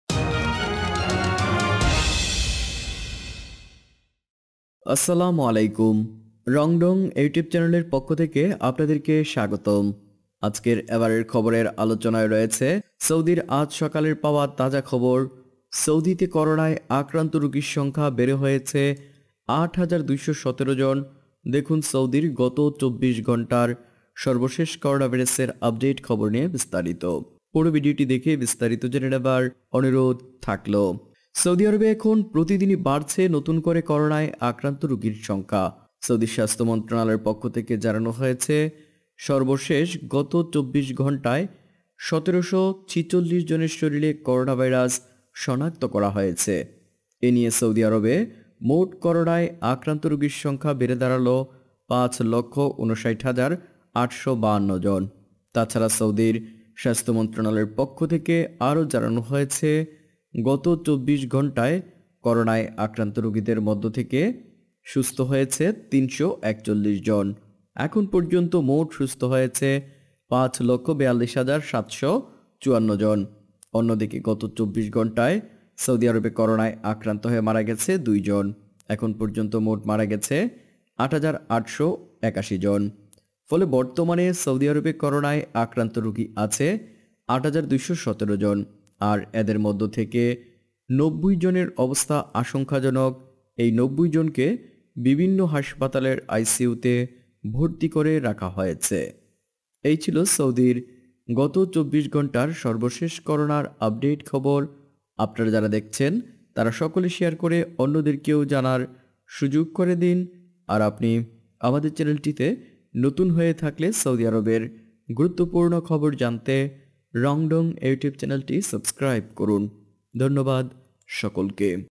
সৌদির আজ সকালের তাজা খবর – রোগীর সংখ্যা বেড়ে হয়েছে ৮২১৭ জন। সৌদির করোনার সর্বশেষ খবর।
এবারের খবরের আলোচনায় রয়েছে সৌদির আজ সকালের তাজা খবর  – রোগীর সংখ্যা বেড়ে হয়েছে ৮২১৭ জন। সৌদির করোনার সর্বশেষ খবর নিয়ে বিস্তারিত। পুরো ভিডিওটি দেখে বিস্তারিত জেনে নেবার  অনুরোধ রইলো।